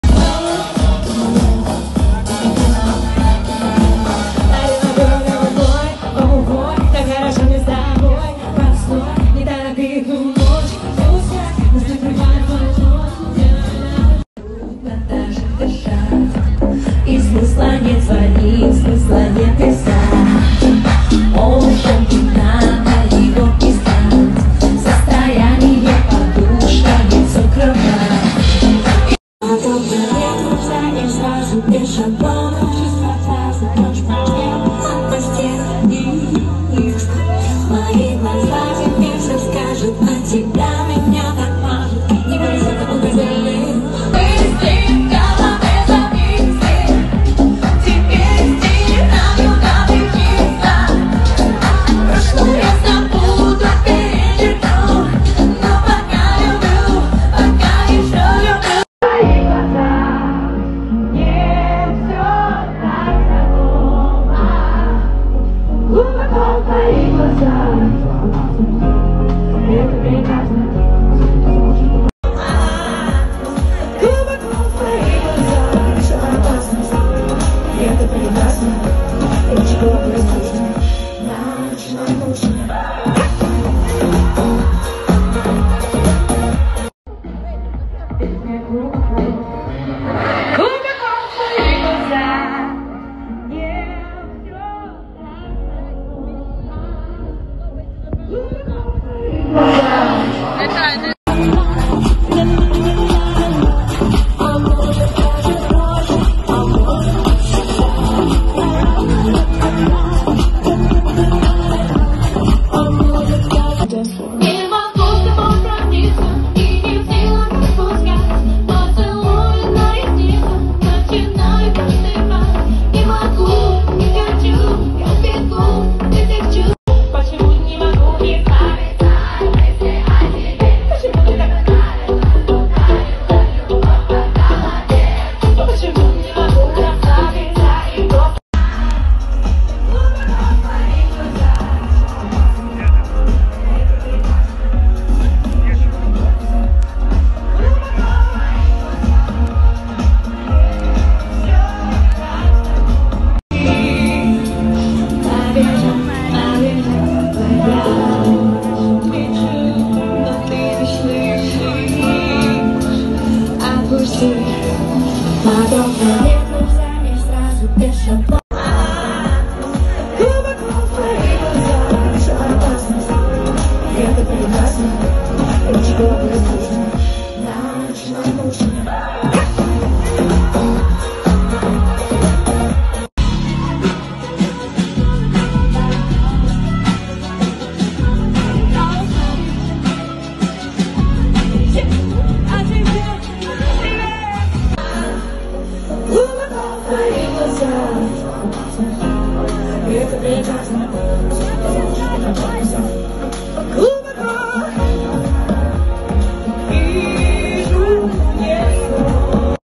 DOROFEEVA UNDERHILL FESTIVAL 🍸 (FanShooting)
Edit by : Sofia Korzun

Смотрите, как прошёл cольный концерт DOROFEEVA на Undrhill Festival 2021! Делимся вами атмосферой этого важного и волнительного дня.
Спасибо всей команде❤️